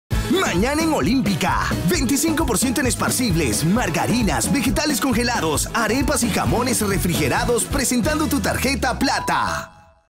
Descuentos 23 de julio
Mañana 25%dcto con Tarjeta Plata en esparcibles, margarinas, vegetales, congelados, arepas y jamones refrigerados. Válido 23 de julio *AC